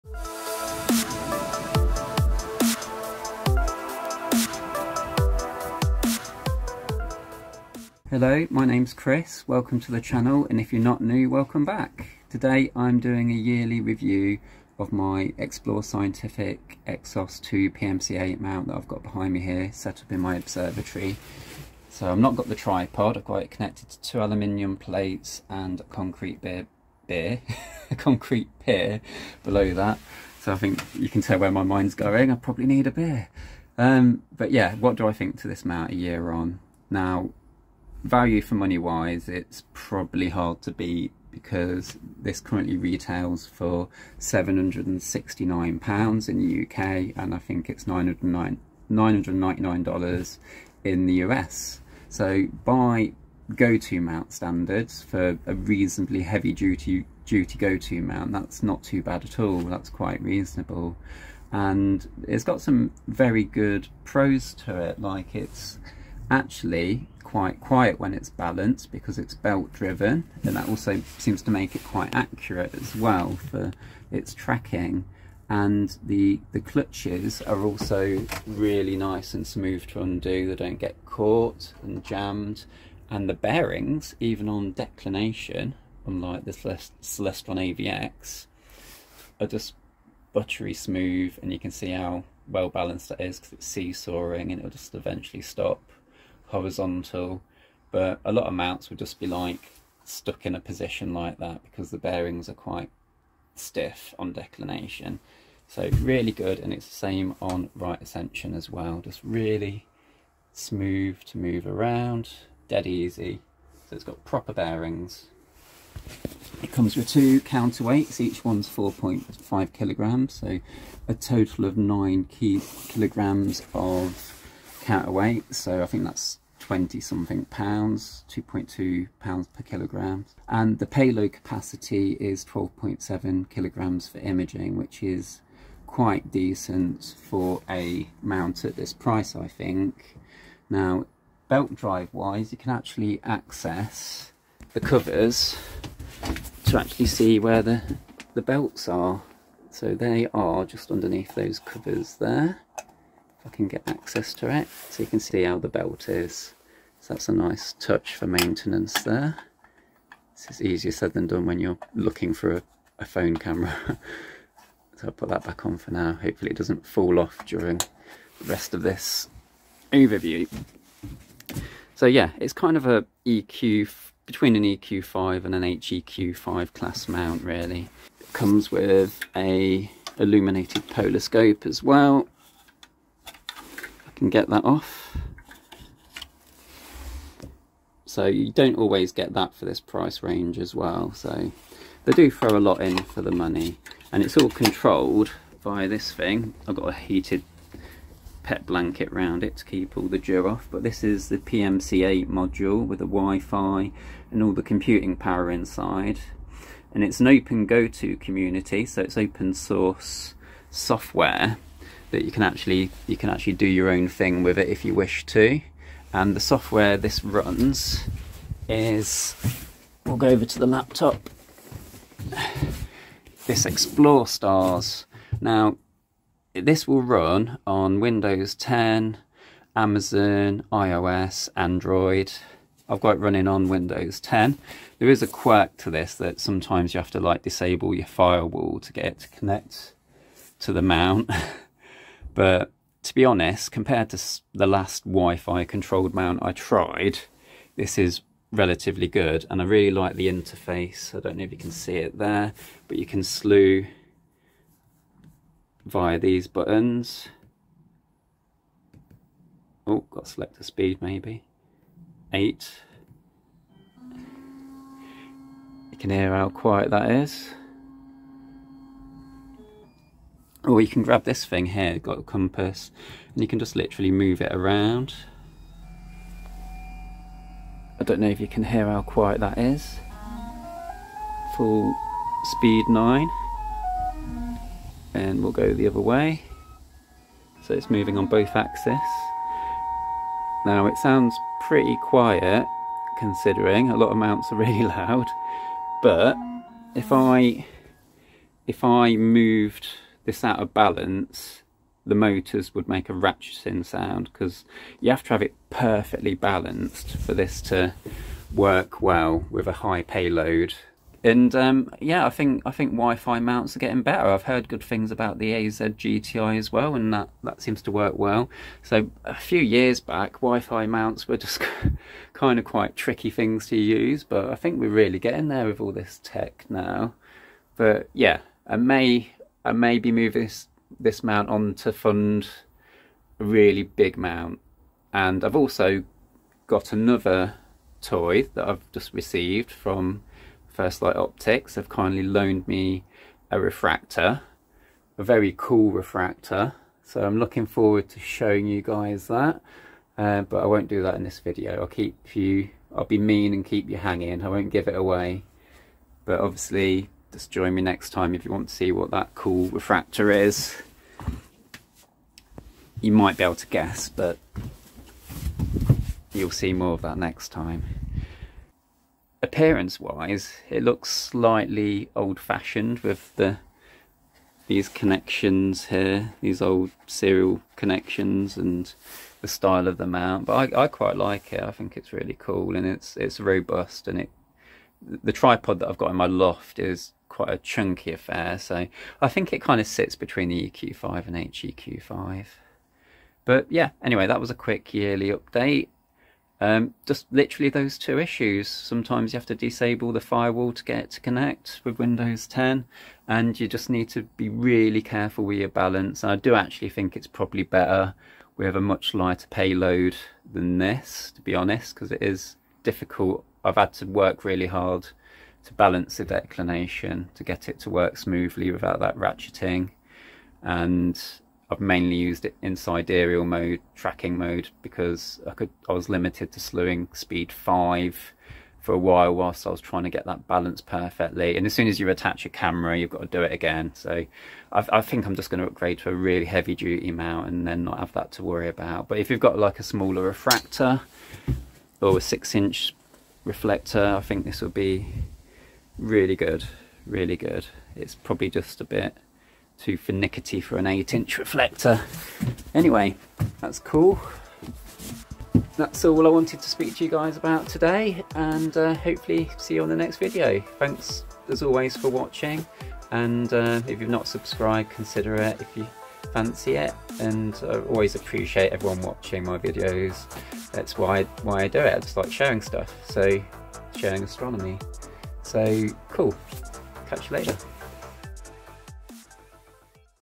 One Year Owning the Explore Scientific EXOS2 pmc-eight GOTO Mount.
This is a quick overall opinion of the EXOS2 pmc8 mount after one year of ownership. I run through the cool features as well as some of the niggles I've experienced. One important thing I meant to include was the goto accuracy. I've never managed to setup accurate goto using the ExploreStar's 2-3 star alignment routine. I believe goto works much better with third party software via ASCOM.

**Beginner Telescopes**

**Telescopes In Stock**

**Offers**

**General Telescope Section**

**Mounts**

**Cameras/Astrophotography**

**Eyepieces**

**Filters**

**Accessories**

**Binoculars**